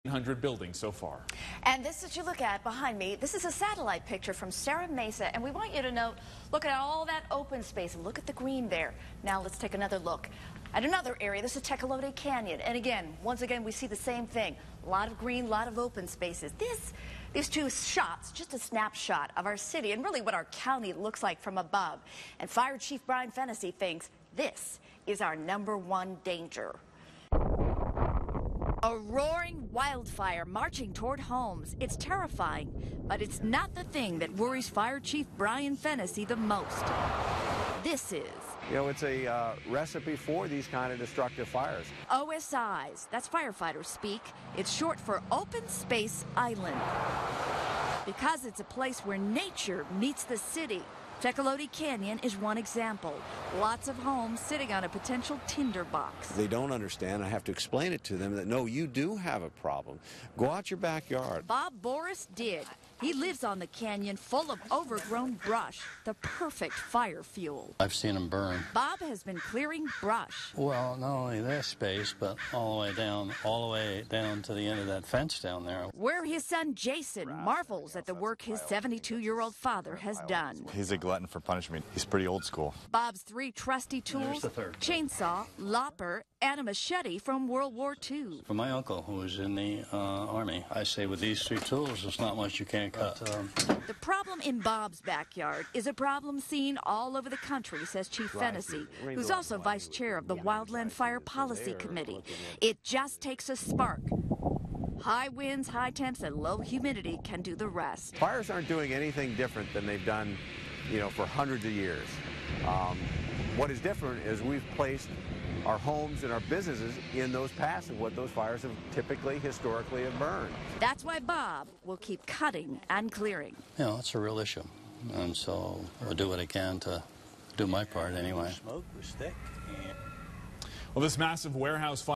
SDFD Chief on wildfire Danger
Chief Fennessy describing the dangers of wildfires within San Diego City.